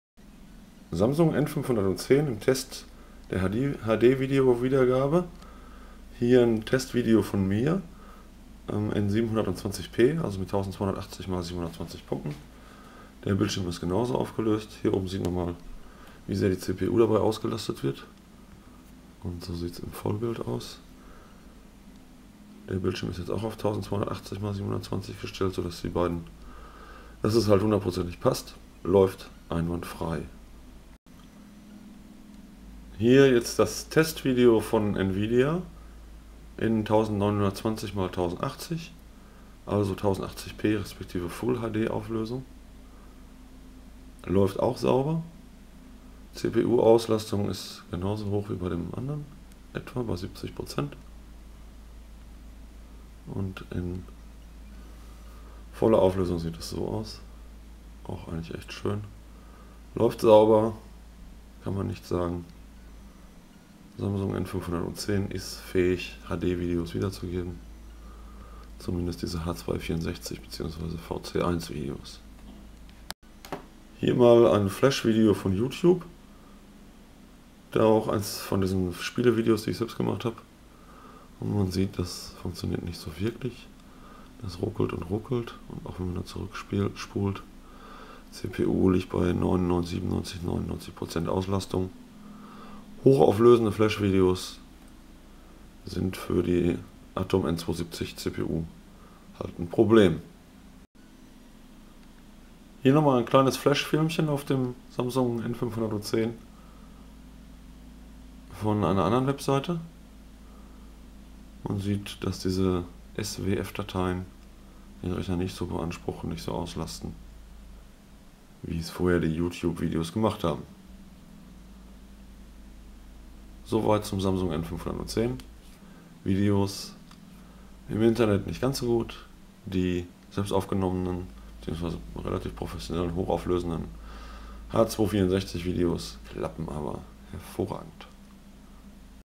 Samsung n510 ion playing full hd
vier verschiedene Tests mit dem Samsung N510 Netbook mit NVidia ION Grafik, 1. 720p 2. 1080p, 3. youtube hd, 4. swf-movie, alles prima ausser youtube hd, 1080p geht wenn dxva videobeschleunigung aktiviert ist,combined community codec pack bringt die lösung...weitere Netbookvideos in meinem account